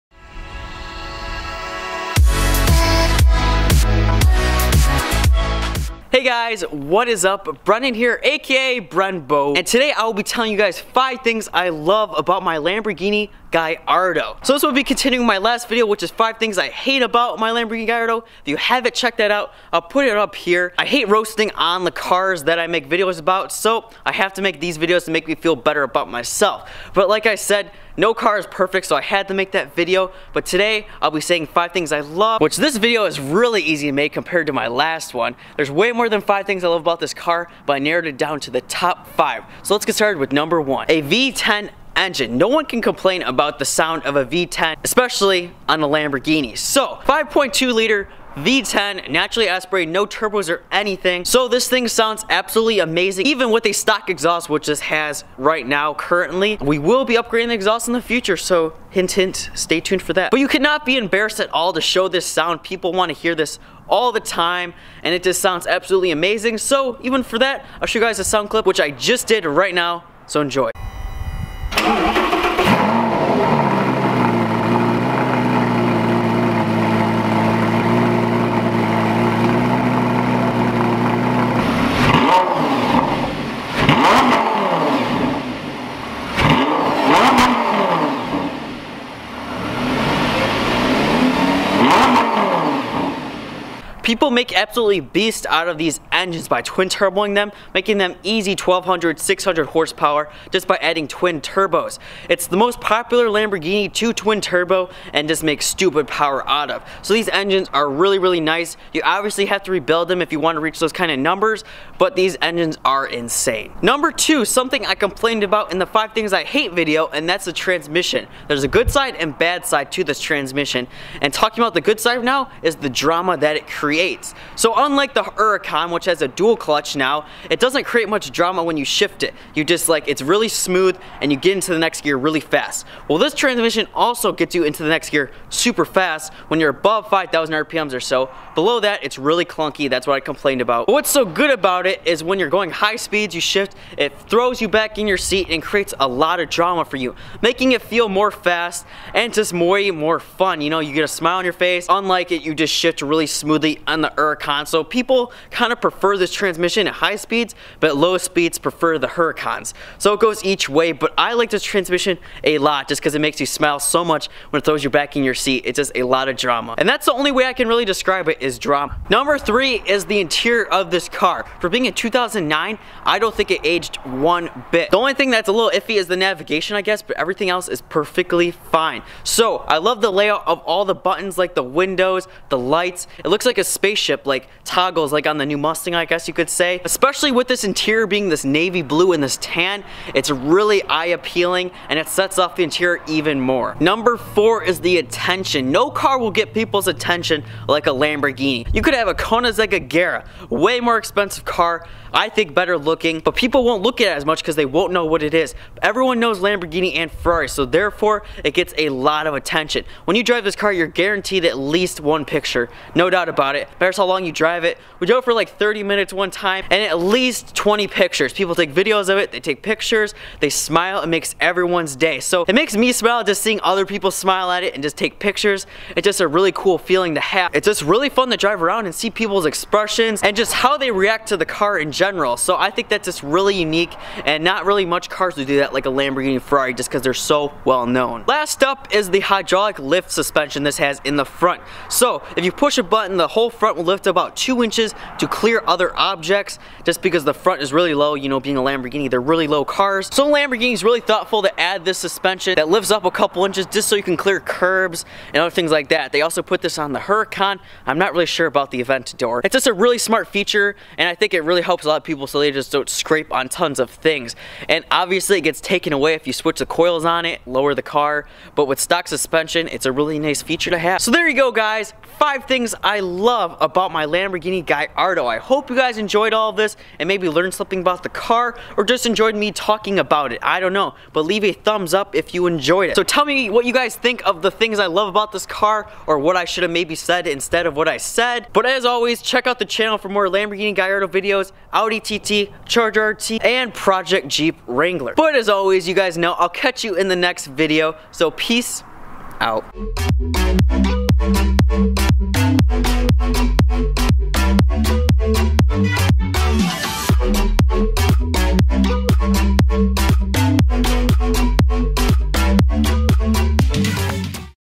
5 Things I LOVE About The Lamborghini Gallardo!
Today I tell you 5 things I love about my Lamborghini Gallardo! I love way more than 5 things about this car but the video would be too long! Join the Bren Bros!

+ Follow me on [PARISCOPE]: Brenbo